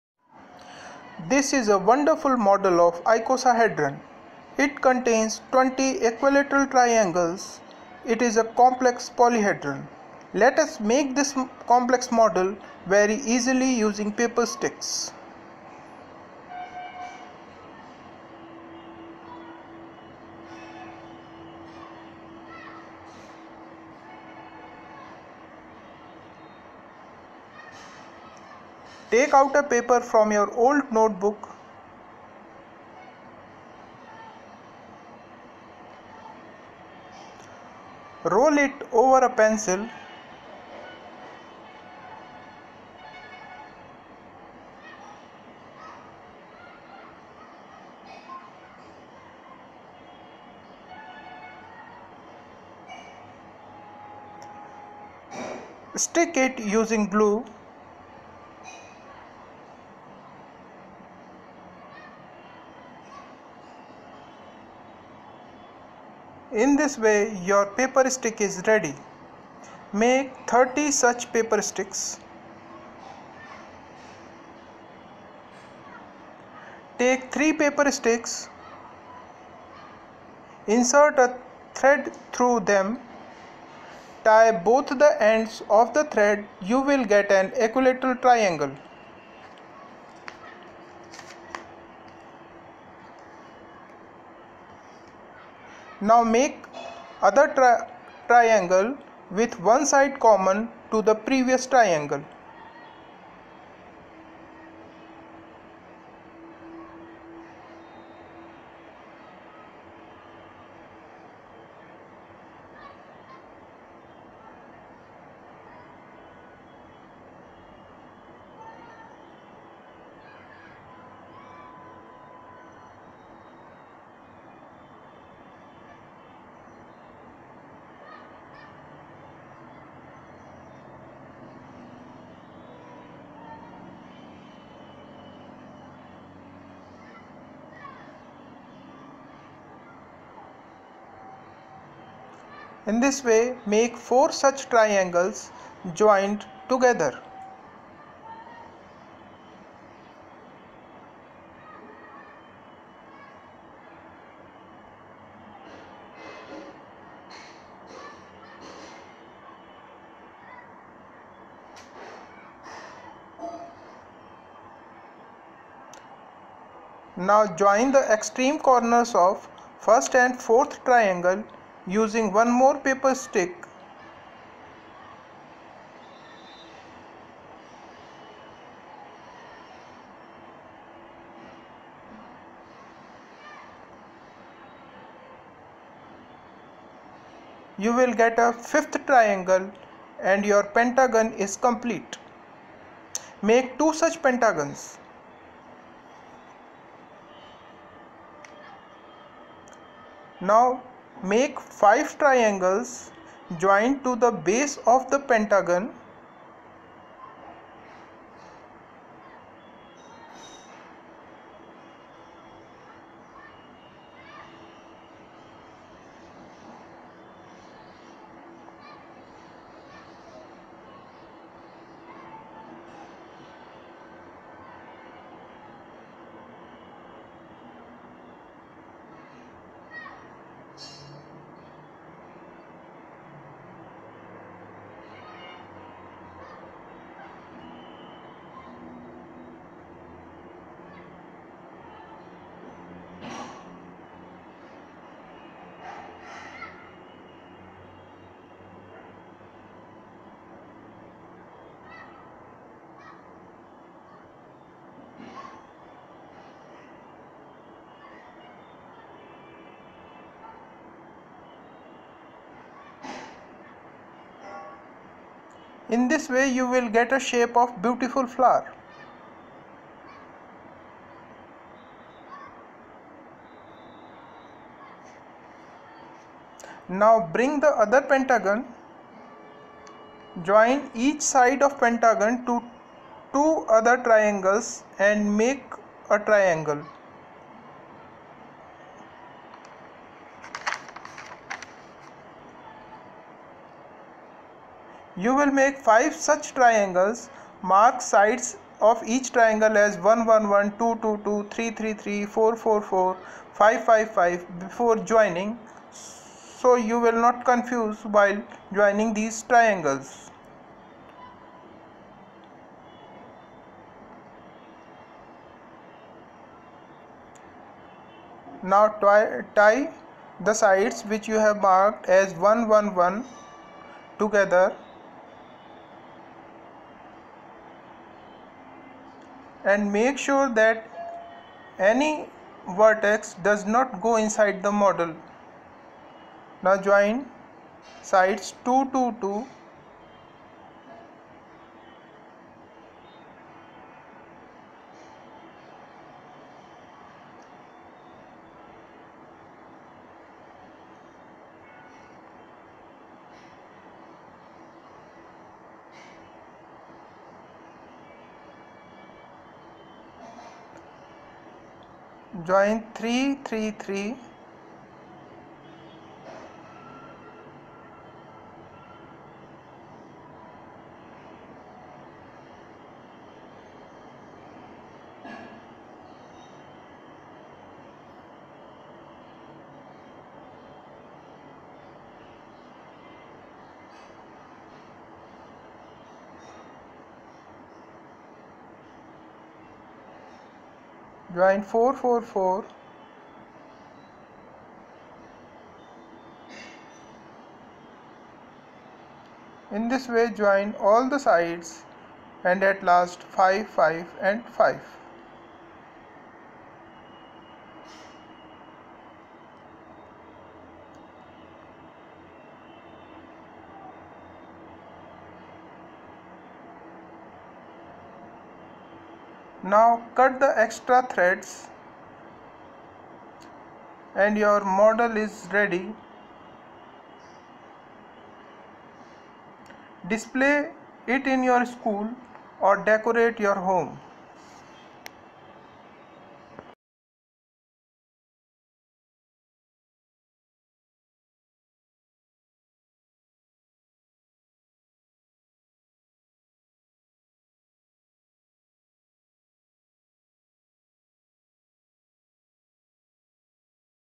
how to make Icosahedron | still paper stick model
Paper stick models are very easy, innovative and low cost. You can make some very complex 3d shapes using this paper stick method. In this video you will see how to make a Icosahedron.